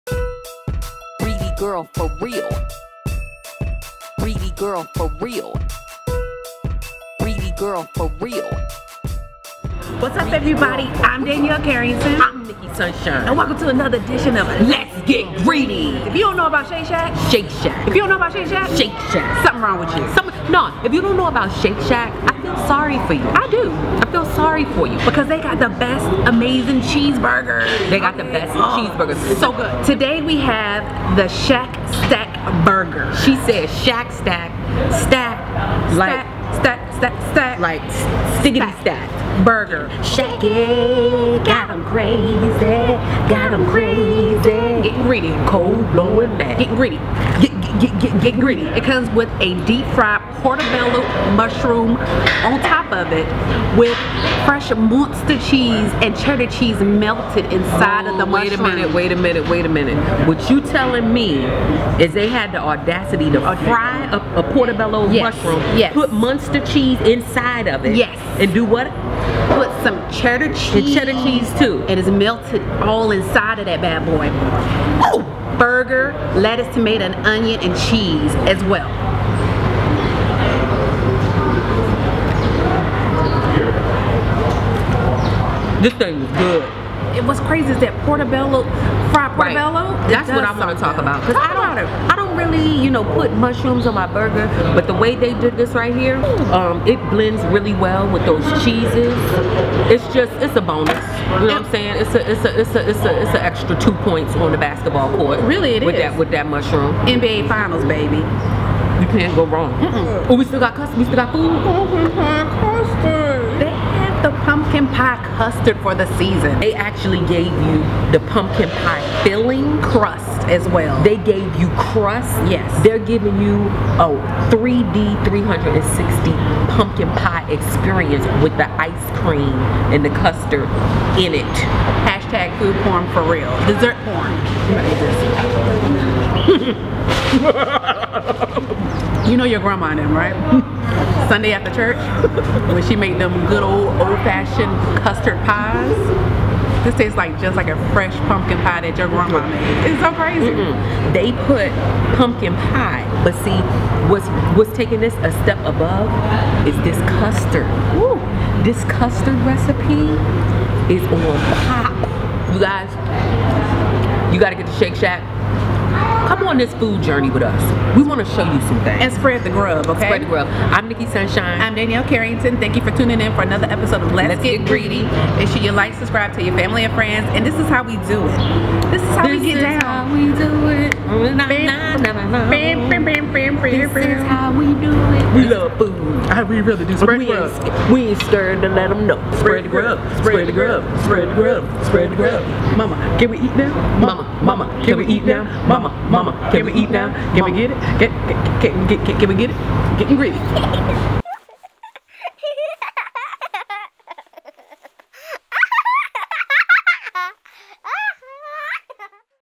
Shake Shack's the "SHACK STACK BURGER'' on Let's Get Greedy! Food Review #70
This episode is about 2 Comedic Food Critics trying a cheeseburger topped with a crispy fried portabella mushroom which is stuffed with muenster & cheddar cheeses, lettuce, tomato, onion and a specialty sauce.  Also, they are tying their pumpkin pie frozen custard.

Featured Eatery:
Shake Shack